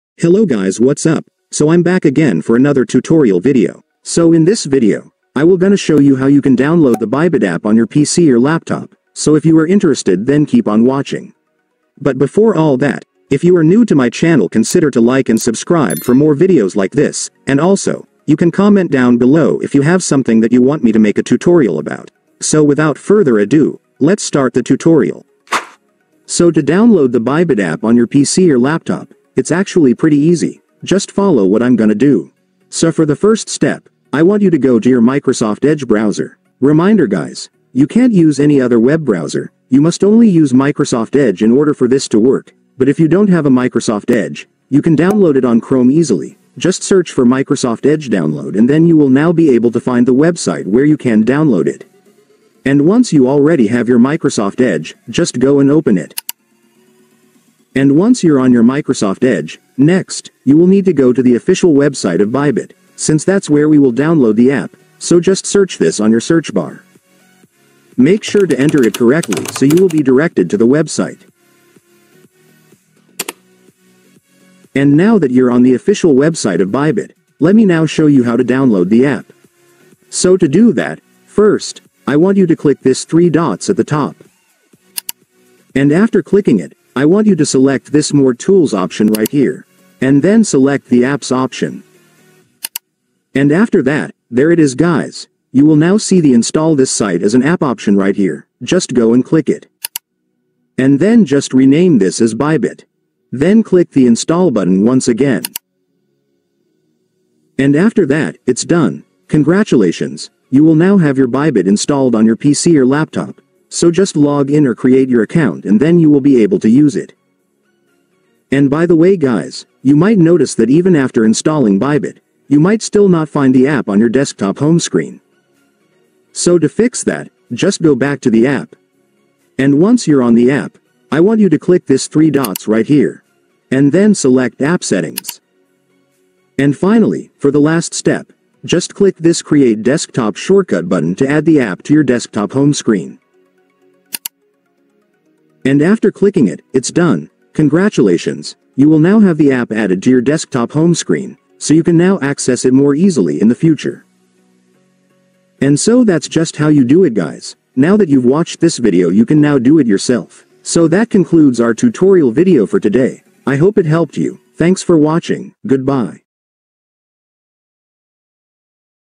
How to Install BYBIT App on Your PC or Laptop - Easy Step-by-Step Guide (2025)
Learn how to download Bybit on pc or laptop in this quick tutorial video.